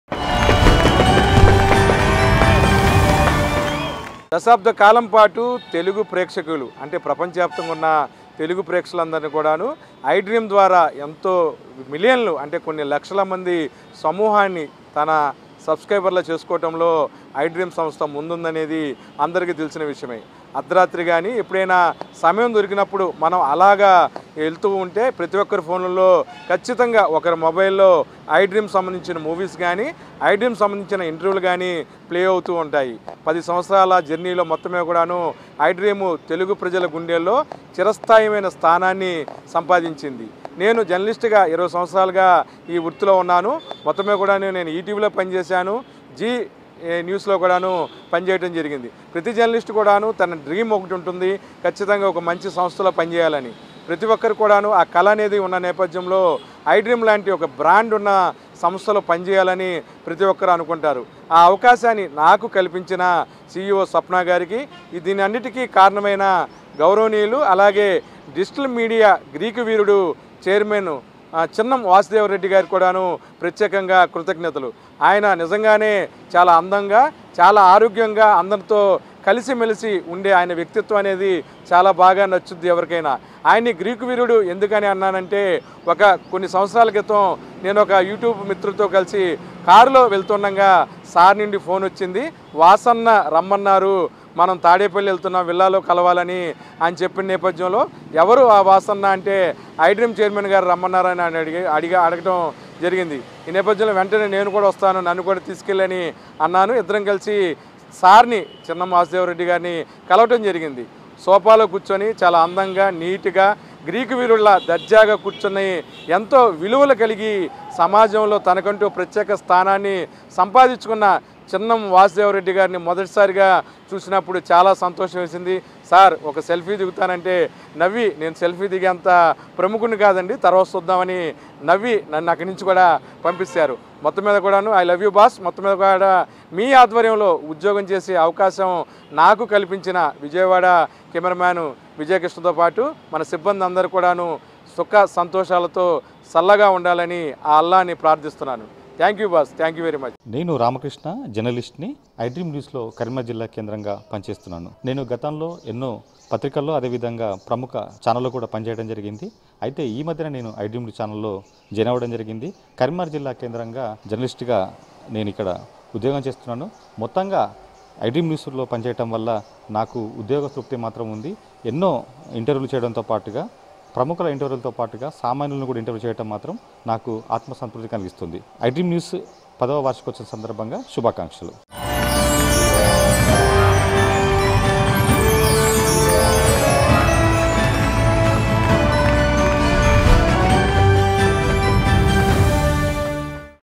దశాబ్దకాలంగా విరాజిల్లుతున్న ఐడ్రీంకి శుభాకాంక్షలు | Reporters Best Wishes | iDream 10th Anniversary
Join us in celebrating iDream Media's 10th anniversary with special messages from iDream Media Reporters Aleem (Vijayawada), Ramakrishna (Karimnagar). They congratulated and showered with wishes for iDream Media's continued success.

Thank you for choosing iDream Media as your source of entertainment for the past decade. We look forward to bringing you more exciting content in the years to come!